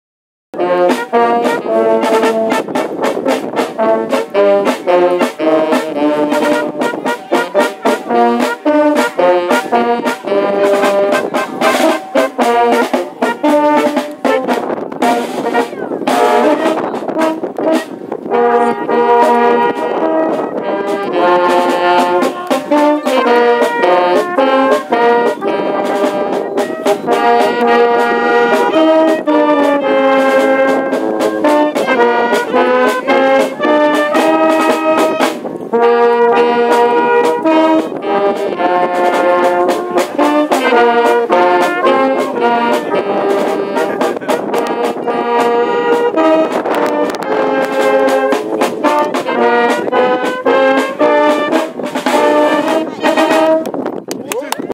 Bavarian Ump-pa-pa Band at Ayers Rock Sunset Party
At the tailgate party awaiting sunset at Ayers Rock, there was a Bavarian band who were on a world tour and played German Beer Hall music for all to enjoy!